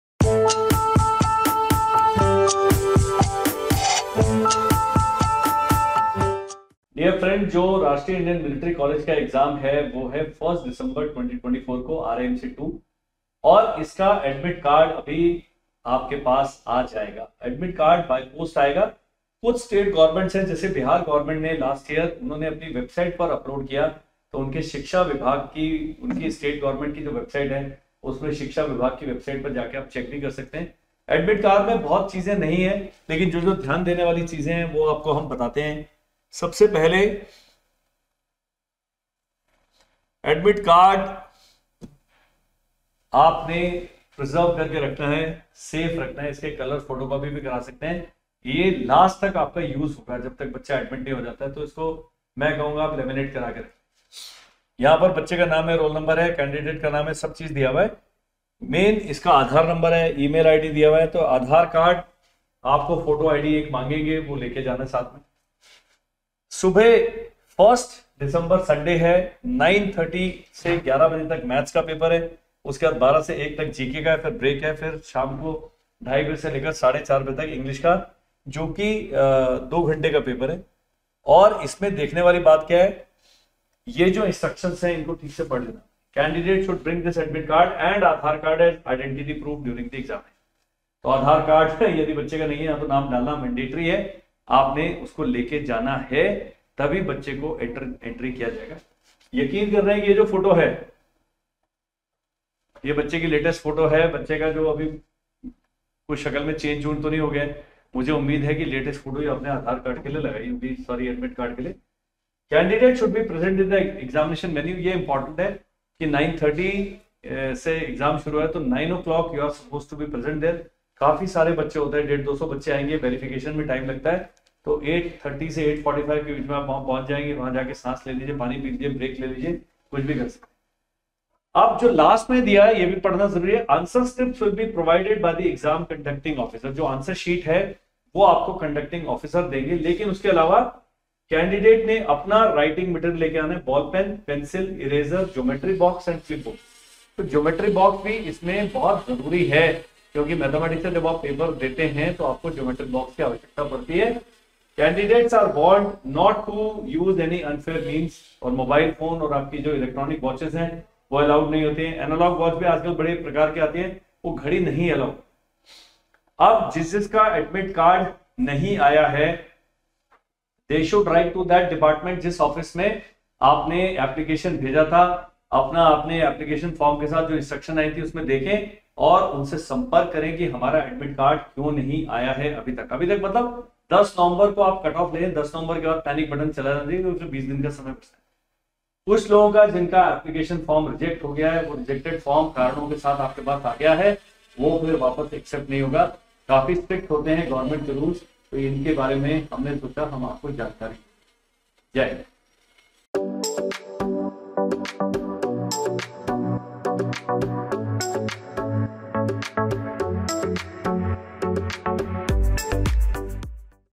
Information about RIMC December 2024 Admit Card and Important Tips
Important Information about RIMC December 2024 Admit Card and Important Tips | RIMC 2024.
How to Download RIMC Admit Card December  2024 | RIMC Admit Card 2024 Download. Students for RIMC December Entrance Exam should keep in mind -
1. Aadhar Card
2 RIMC Admit Card Download and print.
3. RIMC exam papers Time and duration.
4. Reach in time for RIMC Entrance Exam
5 Stationary to be carried for RIMC Exam.
6. Contact If RIMC admit card is not received.

Date for RIMC Entrance Exam December 2024 is 01 December 2024. RIMC Admit card will be received by post however, visit your state government website to search and download RIMC Admit Card for RIMC Entrance Exam December 2024. RIMC admit card for Bihar students appearing for RIMC Entrance Exam December 2024 is available on Bihar government website to download online.
All the best for students appearing for RIMC Entrance Exam December 2024.